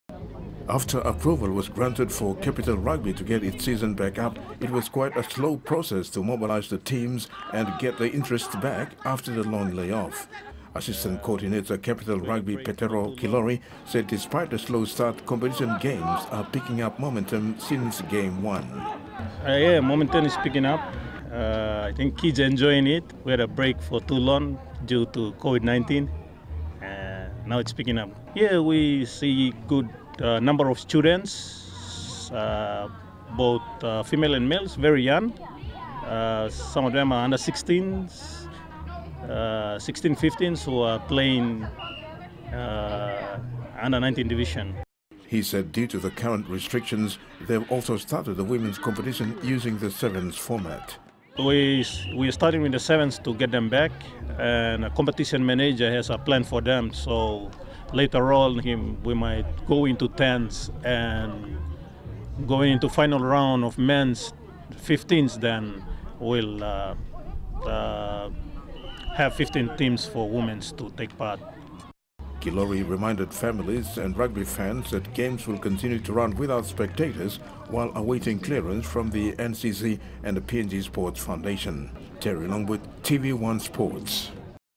Slow Start To Capital Rugby
Capital Rugby Union staged its 4th week of 15s competition today at the Bava Park.

The games are being played with no spectators as per the NCC and PNGSF Covid-19 conditional approval for the competition to start.